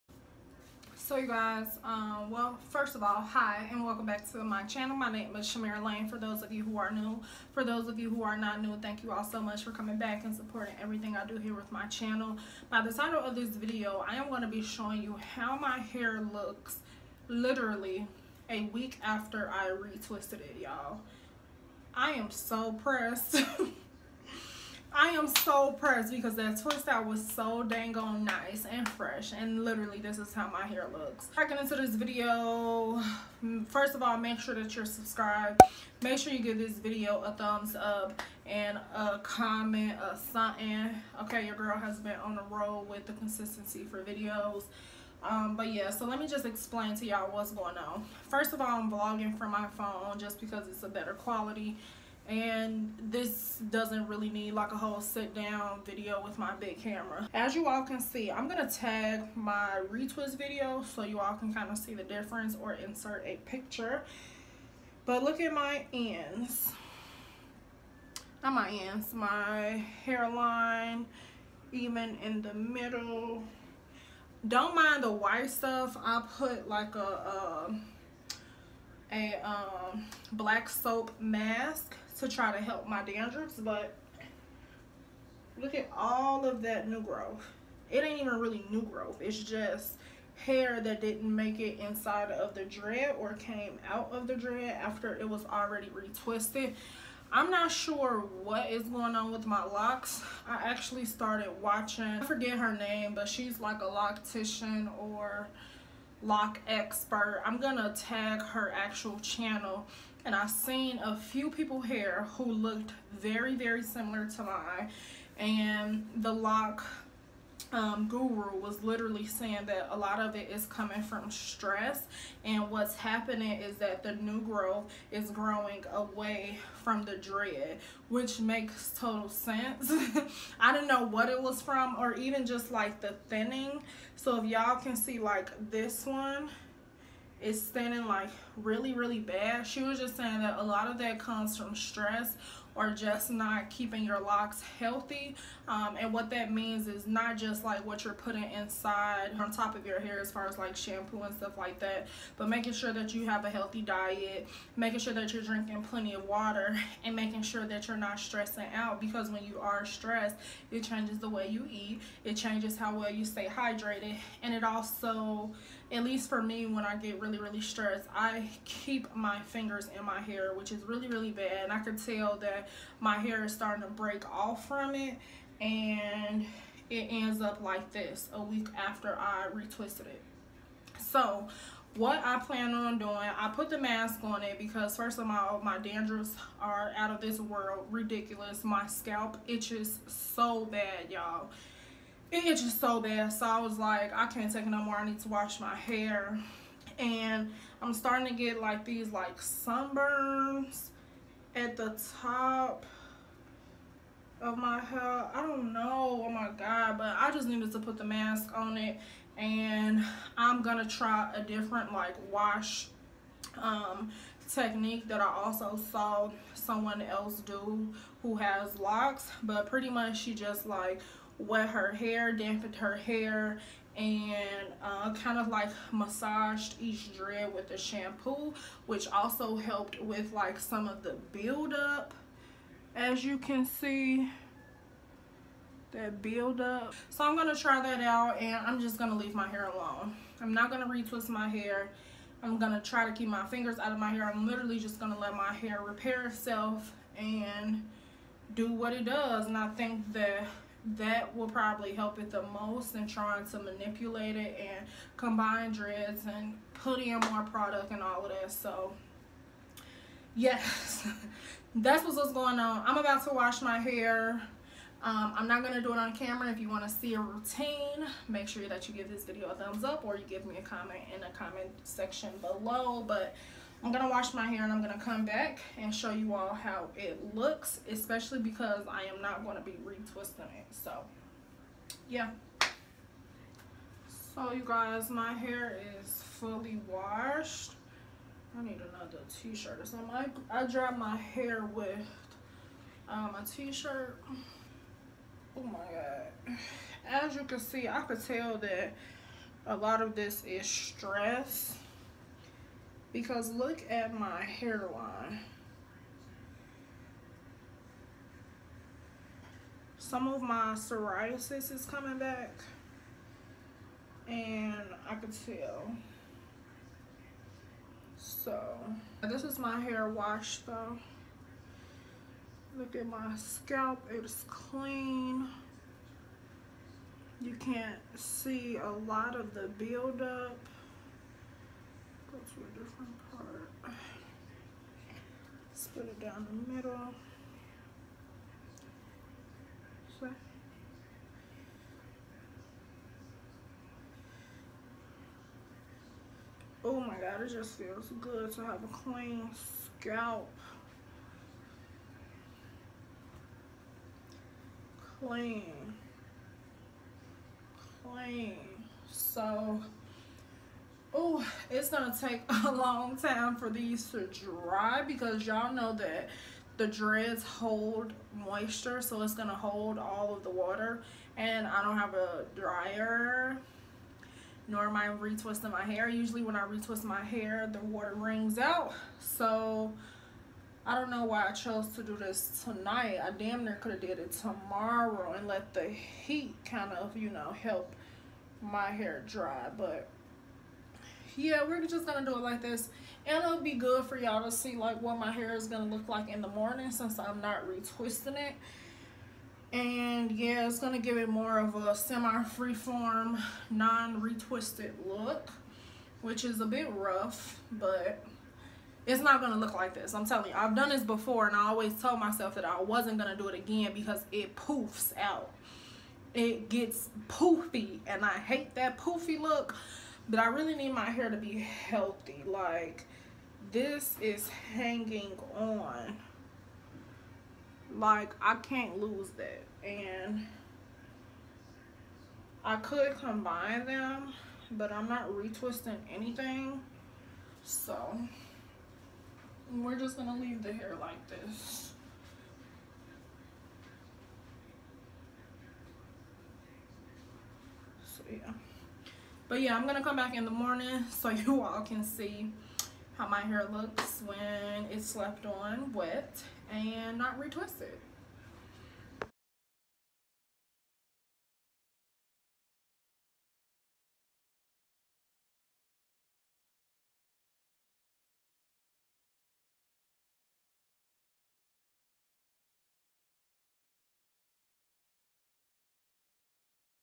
LOC RETWIST | 1 WEEK LATER 😩
Hey y'all! This is what happened to my re-twist a week after I did them lol. A lot of this is due to stress, the humidity happening in Chicago and not oiling my scalp as frequently as I should. So I re-washed it more thoroughly and decided not to re-twist it so that I wasn't pulling my hair - I wanted to just let it be. Like this video for my in depth wash routine!